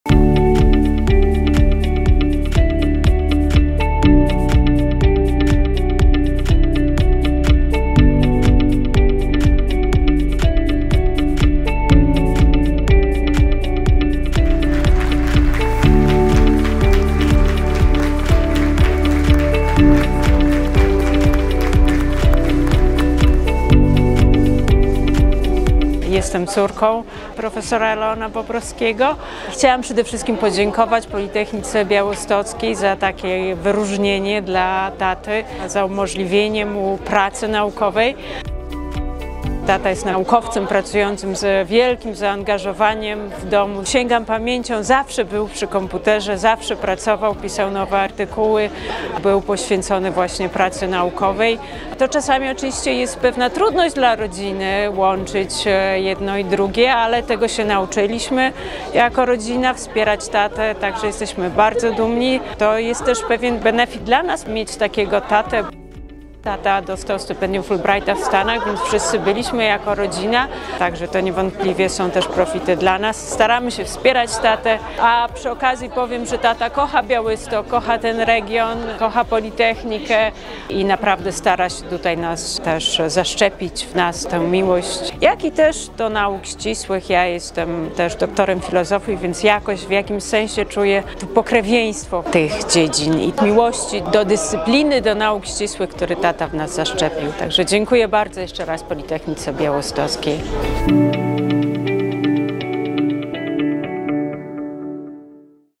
Ewa Bobrowska o nadaniu tytułu Honorowego Profesora PB prof. Leonowi Bobrowskiemu
14 czerwca 2024 r. podczas Gali Jubileuszowej Wydziału Informatyki Politechniki Białostockiej tytuł Honorowego Profesora Politechniki Białostockiej otrzymał prof. dr. hab. Leon Bobrowski – wybitny naukowiec i światowy autorytet w zakresie eksploracji dużych zbiorów danych biomedycznych, mentor pracowników naukowych, nauczyciel i wychowawca studentów.

Jego córka, dr Ewa Bobrowska podkreśla znaczenie tego wyróżnienia dla całej rodziny Pana Profesora. – Jesteśmy bardzo dumni. Tym bardziej, że Tata kocha Białystok, ten region i Politechnikę Białostocką. Zawsze starał się zaszczepić w nas tę miłość, również do nauk ścisłych. Odkąd sięgam pamięcią, zawsze był przy komputerze, zawsze pracował, pisał nowe artykuły, był poświęcony pracy naukowej – mówi dr Ewa Bobrowska.

Dziękujemy za te słowa i wspieranie prof. Leona Bobrowskiego w pracy na rzecz Wydziału i naszej Uczelni. Dzięki jego wizji w regionie północno-wschodnim powstał silny ośrodek badań naukowych oraz dydaktyki informatyki. Pan Profesor kierował nim przez w sumie 11 lat, jako dziekan Wydziału Informatyki w kadencjach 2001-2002, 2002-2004, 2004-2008 i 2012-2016.

Uroczystość nadania prof. Leonowi Bobrowskiemu godności Honorowego Profesora Politechniki Białostockiej była jednym z elementów Jubileuszu 75-lecia Politechniki Białostockiej